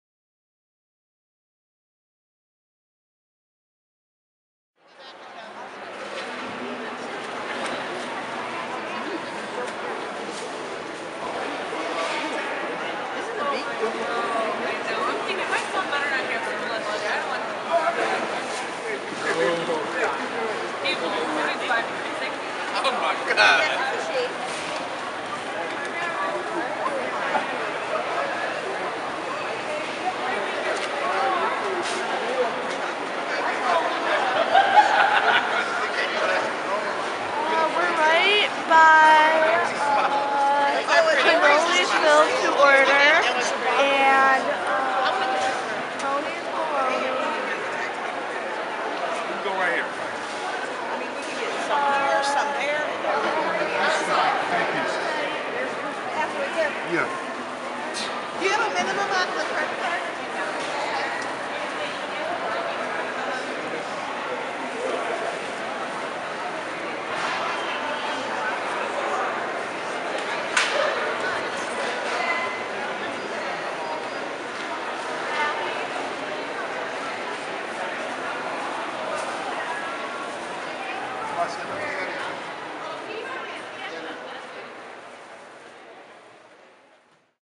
Westside Market
Me, Sherry, Colleen and Leah strolling the amazing place known as the Westside Market in Cleveland. If you listen closely you can hear me drooling.